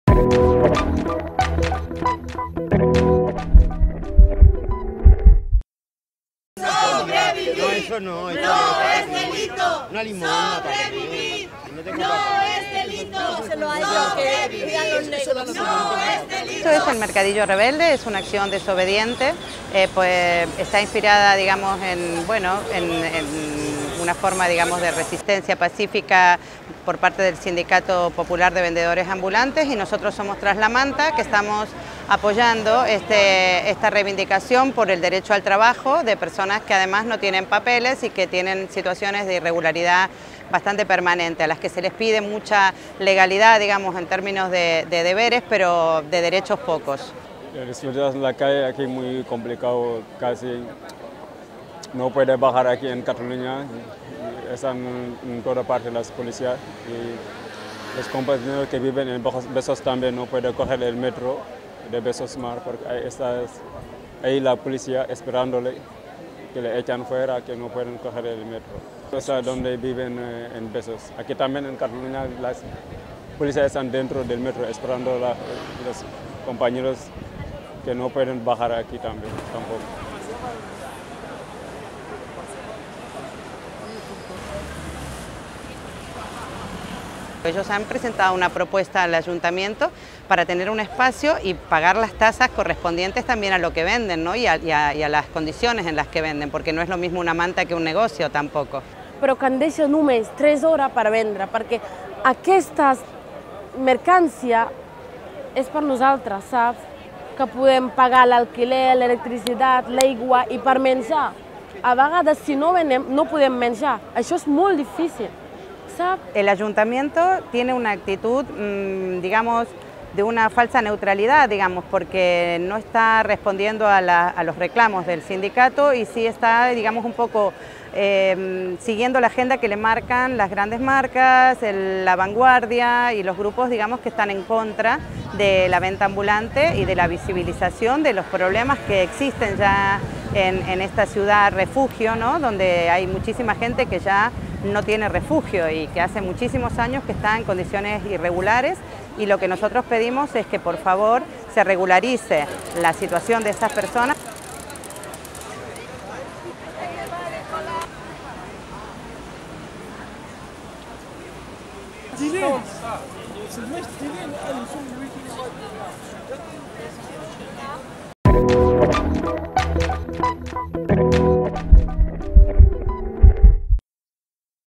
Els manters denuncien que la Guàrdia Urbana els impedeix entrar al Metro
Redacció: Jesús Rodríguez
Audiovisuals: Marta Perez i Sònia Calvó
Fotografia: Gorka Leiza

Els agents se situen des de fa tres setmanes als accessos de l'estació Besòs Mar i escorcollen els joves d'origen subsaharià que intenten accedir al transport suburbà amb farcells o motxilles.

El 19 d'abril va arrencar una operació policial coordinada entre la Guàrdia Urbana, els Mossos d'Esquadra i els agents de seguretat privada del Metro de Barcelona per a impedir la presència de venedors ambulants al vestíbul i les andanes de l'estació de plaça Catalunya. Lamine Sarr, portaveu del Sindicat Popular de Venedors Ambulant, va revelar ahir en el marc d'una nova edició del Mercat Rebel a Canaletes que els agents, a més, impedeixen que els manters puguin traslladar-se en Metro. “No ens deixen entrar, hi ha policies que ens aturen i no podem entrar al Metro”, va denunciar. Diversos testimonis i afectats han confirmat a la Directa que des del passat 19 d'abril un dispositiu d'agents de la Guàrdia Urbana s'ha instal·lat als accessos de l'estació de Metro Besòs Mar, al districte de Sant Martí de Barcelona. En aquest barri, a banda i banda del carrer Cristòbal de Moura, és on viuen –en condicions molt precàries– la majoria de venedors ambulants d'origen senegalès que treballen al centre de Barcelona.